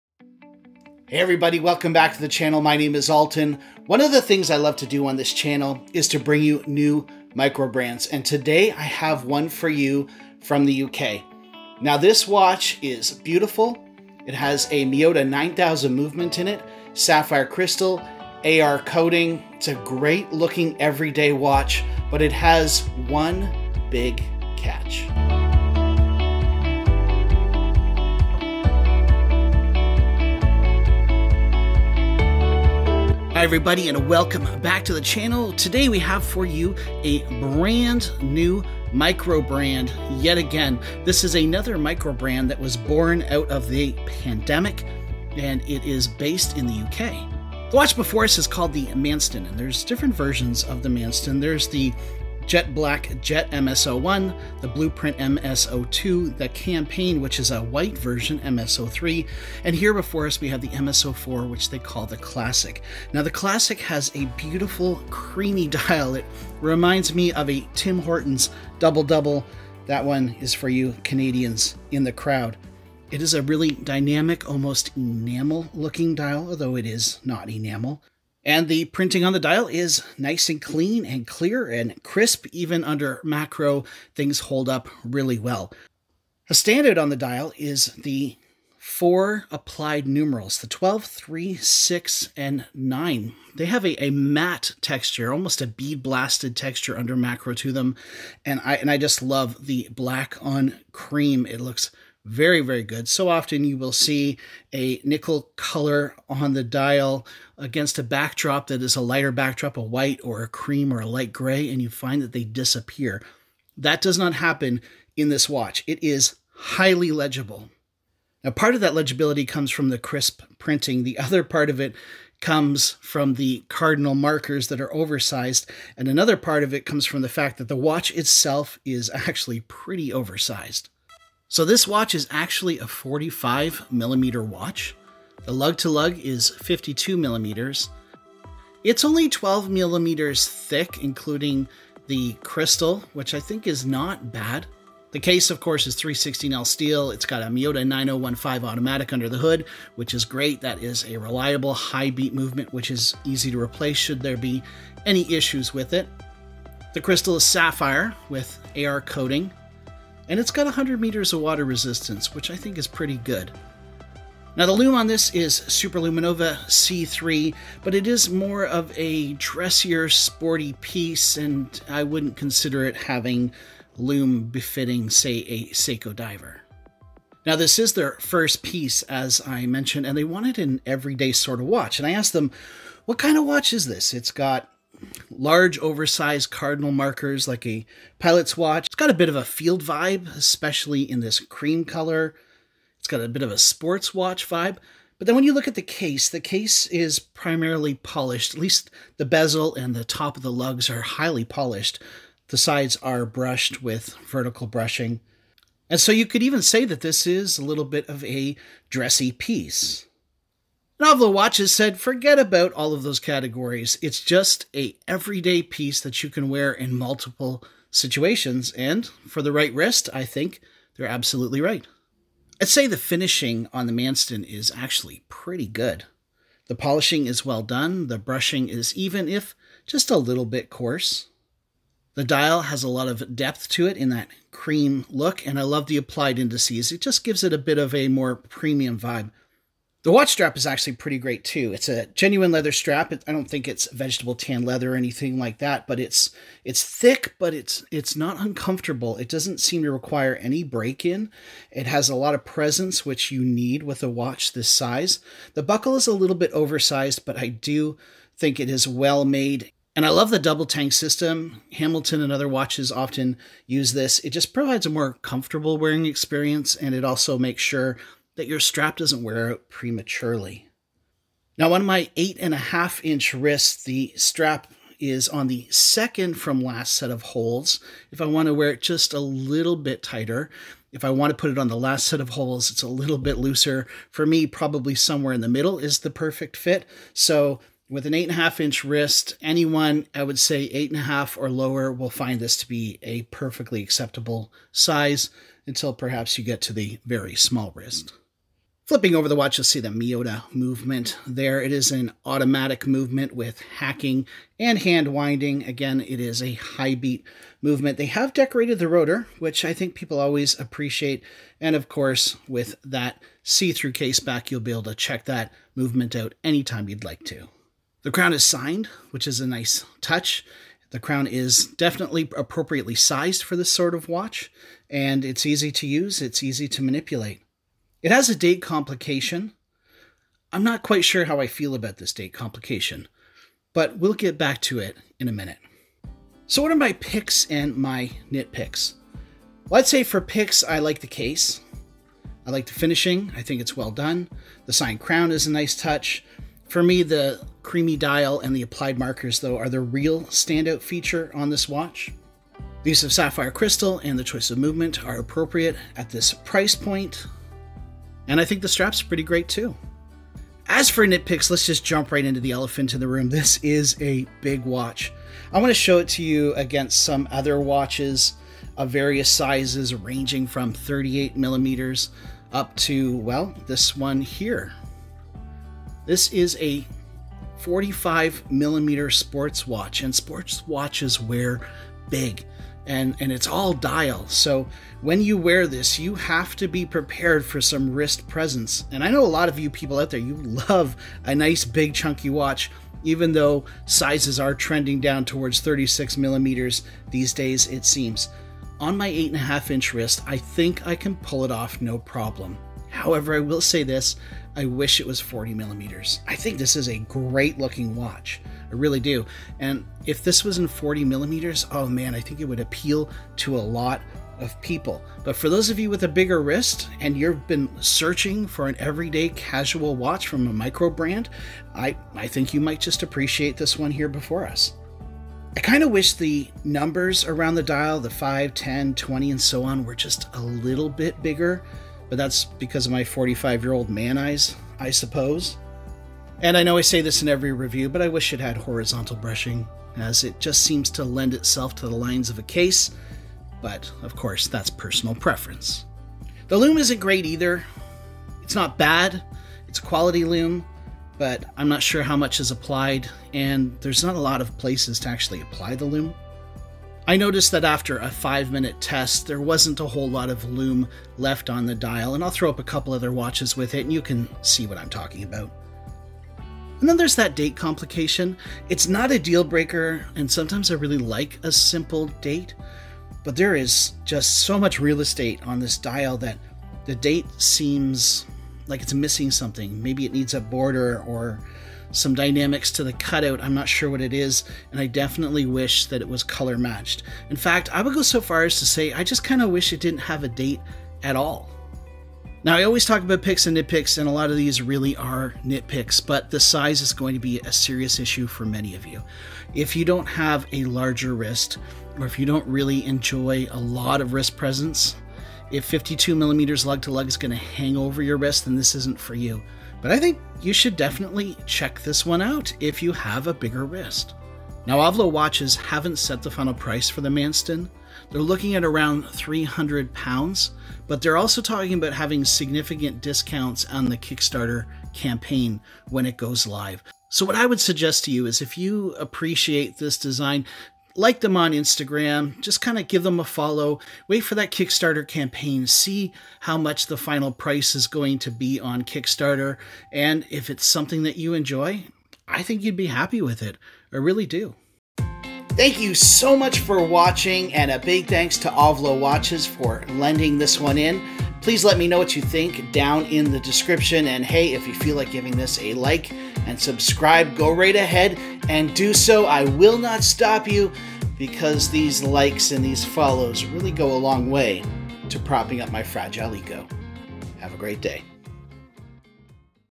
UK NEWCOMER OVLO WATCHES MANSTON PROTOTYPE
Make sure to turn up your settings to highest resolution to take advantage of HD content.

A big thanks to OVLO Watches for loaning this in to the channel for review.

Kickstarter goes live October 25, 2022